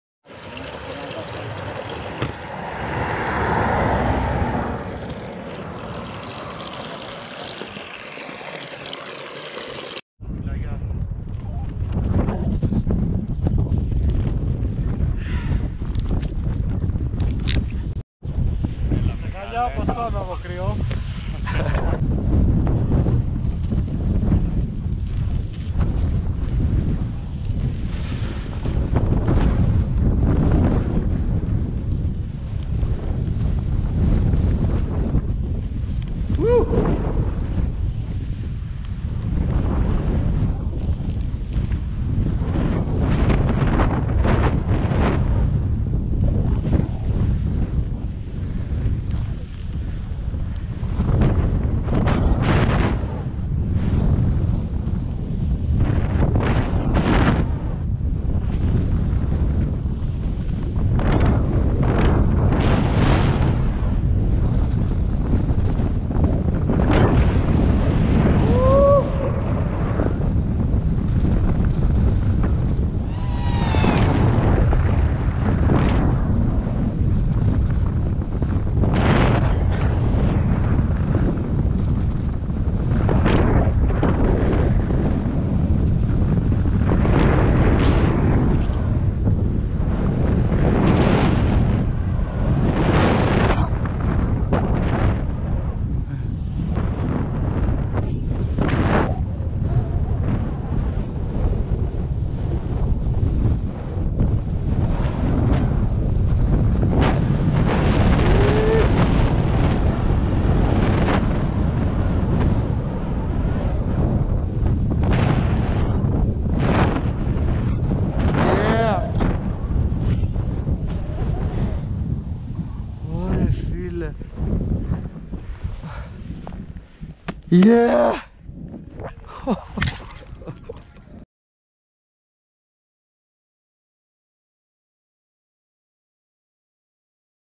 Riding Xaros Line....
Riding The Xaros Slop In The Freeride Contest 2022. After The Contest...

Spot:Kalavrita Ski Center

Rider: Kapetangaridas

Cam:Kapetan Garidas+Black Edition (Gopro Hero7 Black)

Εντιτινγκ:garidas